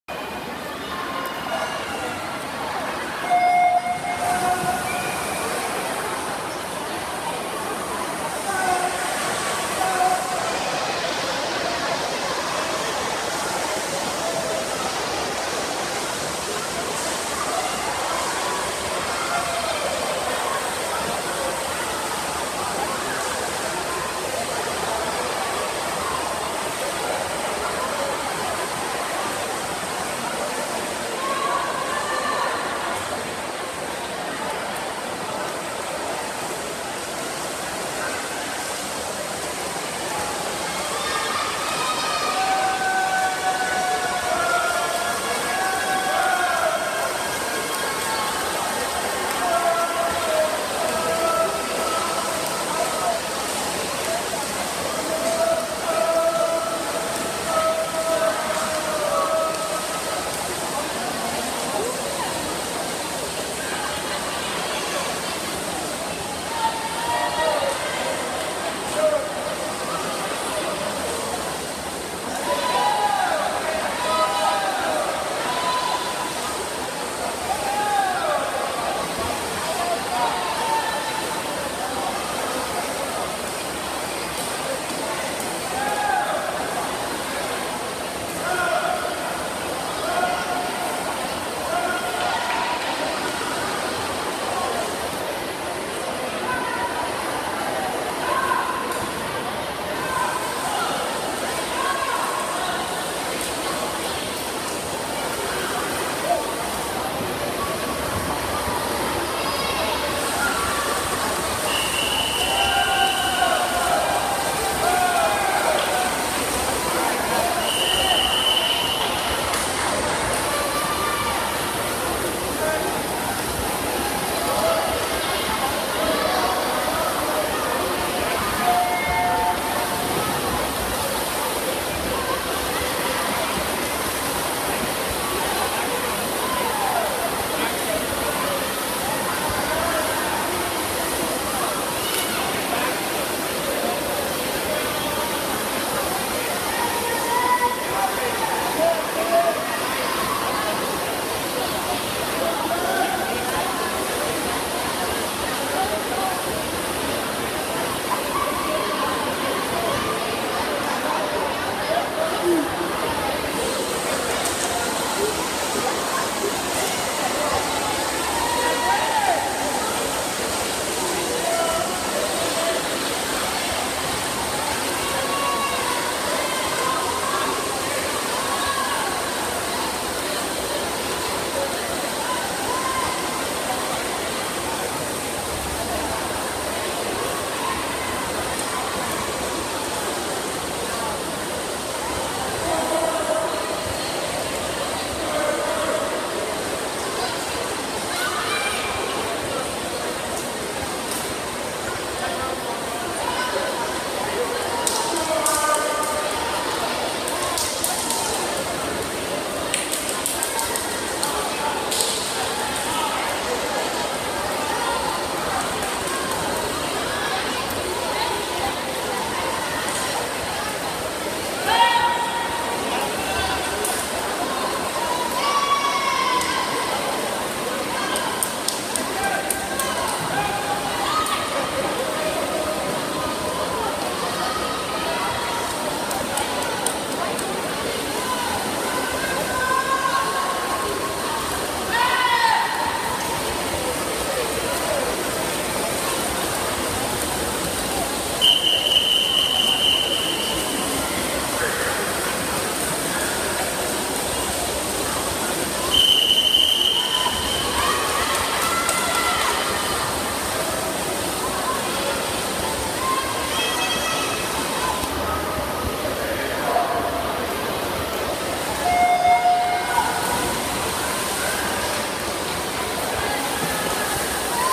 2016 Aggieland April Open Men's 200 IM Heats 1 and 2
Heat 1: 1-Castillo, 2-Bonetti, 3- Tybur, 4- Martinez, 5- Van Overdam, 6- Duplechain                      Heat 2: 3- Kerr, 5- Mulleady, 6- Wong